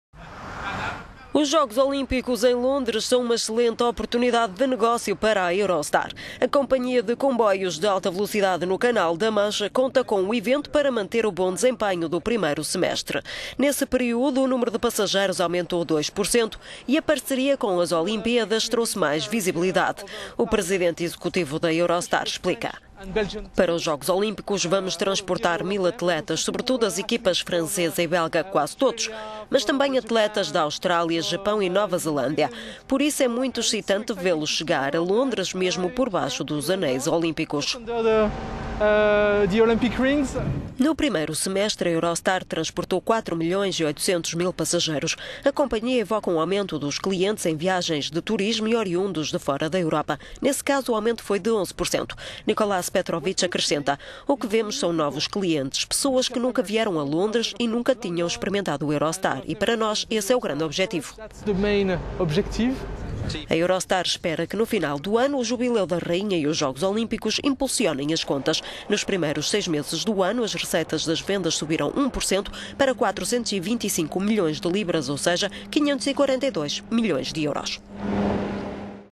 Eurostar conta com JO para acelerar atividade
Os Jogos Olímpicos em Londres são uma excelente a oportunidade de negócio para a Eurostar.

A companhia de comboios de alta velocidade no Canal da Mancha conta com o evento para manter o bom desempenho do primeiro semestre. Nesse período, o número de passageiros aumentou 2% e a parceria com as Olimpíadas trouxe mais visibilidade.

O presidente executivo da Eurostar, Nicolas Petrovic, explica: "Para os Jogos Olímpicos vamos transportar mil atletas, sobretudo, as equipas francesa e belga, quase todos os membros, mas também atletas da Austrália, Japão e Nova Zelândia. Por isso, é muito excitante vê-los chegar a Londres, mesmo por baixo dos Anéis Olímpicos".

No primeiro semestre, a Eurostar transportou quatro milhões e 800 mil passageiros. A companhia evoca um aumento dos clientes em viagens de turismo e oriundos de fora da Europa, sobretudo Estados Unidos e Oriente. Nesse caso, o aumento foi de 11 por cento.

Nicolas Petrovic acrescenta: "O que vemos são novos clientes, pessoas que nunca vieram a Londres e nunca tinham experimentado o Eurostar. Para nós esse é o grande objetivo".

A Eurostar espera que no final do ano o Jubileu da Rainha e os jogos Olímpicos impulsionem as contas.

Nos primeiros seis meses do ano, as receitas das vendas subiram 1% para 425 milhões de libras, ou seja, 542 milhões de euros.